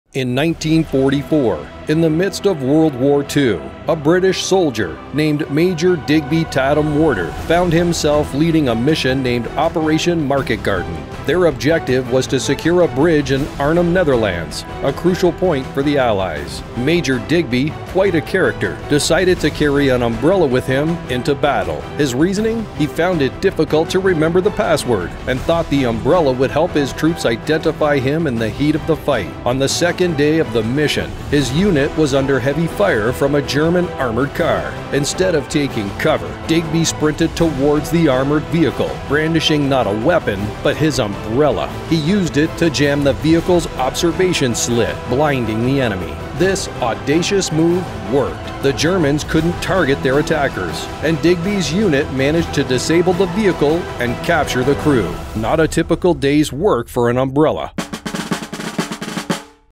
Unbelievable! Man Vs. Armored Car - with an Umbrella?! 😱 WWII Story
💥You WON'T BELIEVE this INSANE True Story from WWII! 💥

Prepare to be amazed as we dive into one of the most incredible, unbelievable tales of audacity and courage from the trenches of World War II - A story of a man, Major Digby Tatham-Warter, and his trusty UMBRELLA going up against a German Armored Car. YES, you read it right - an UMBRELLA! 🌂🔥

Watch the video to know how this soldier turned the tide of an intense battle, not with a machine gun, but with a common household item we wouldn't dream of associating with war. You wouldn't want to miss this epic tale of audacity that has managed to stand the test of time! 🌍⏳